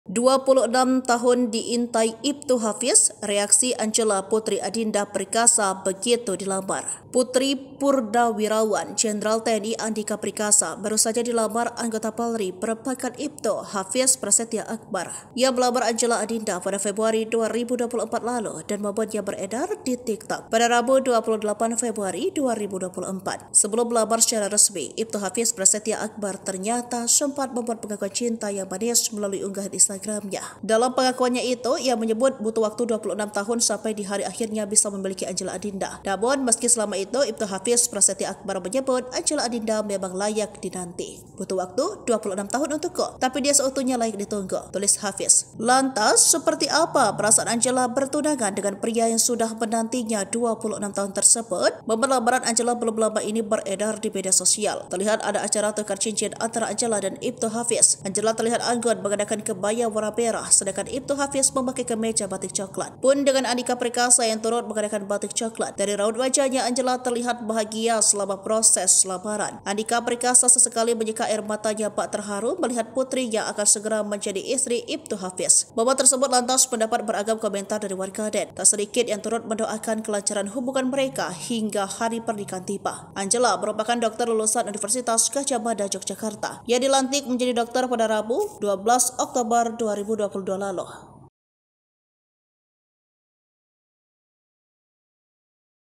26 Tahun Diintai Iptu Hafiz, Reaksi Angela Putri Adinda Perkasa Begitu Dilamar
Purti purnawirawan Jenderal TNI Andika Perkasa baru saja dilamar anggota Polri berpangkat ‭Iptu, Hafiz Prasetia Akbar.

Ia melamar Angela Adinda pada Februari 2024 lalu dan momennya beredar dari Tiktok @xyzsaaa_1, Rabu (28/2/2024).

Sebelum melamar secara resmi, Iptu Hafiz Prasetia Akbar ternyata sempat membuat pengakuan cinta yang manis melalui unggahan Instagramnya.

Palembang.tribunnews.com

Sumber Video: localpridegarage
Editor Video: frizeky
Vo: felin